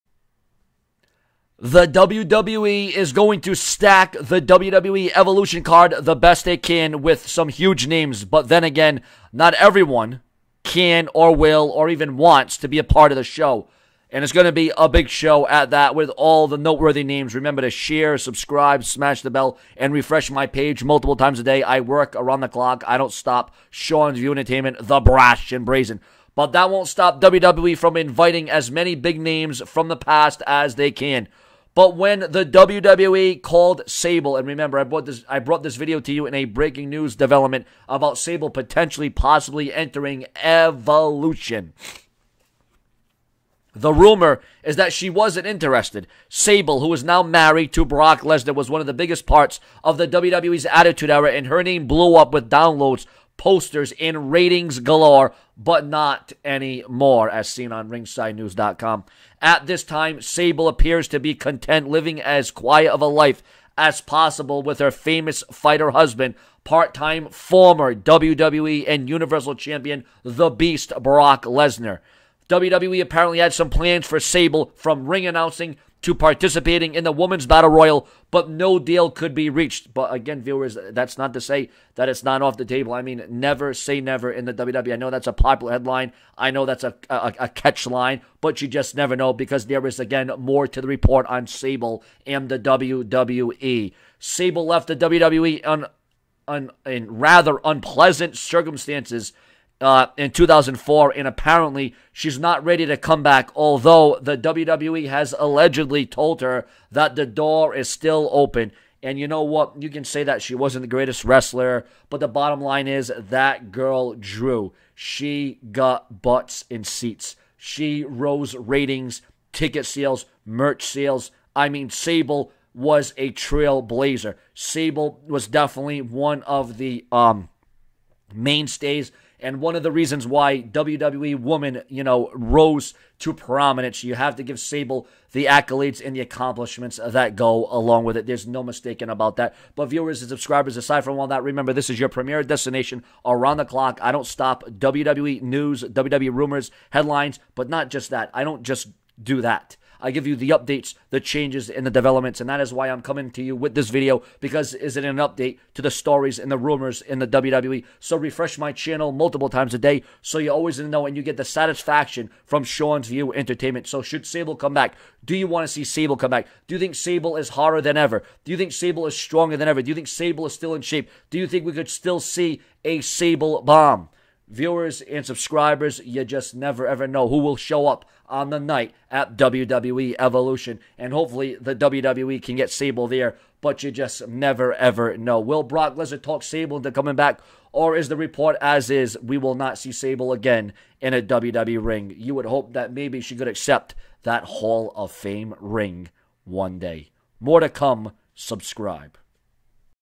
WWE SABLE RUMORED TO HAVE TURNED DOWN WWE EVOLUTION INVITE
SABLE RUMORED TO HAVE TURNED DOWN WWE EVOLUTION INVITE
- Seanzviewent DELIVERS fans, All news, rumors, headlines AROUND THE CLOCK CONSISTENT to GIVING you all the news in the Pro Wrestling World

Facebook.com/seanzviewent/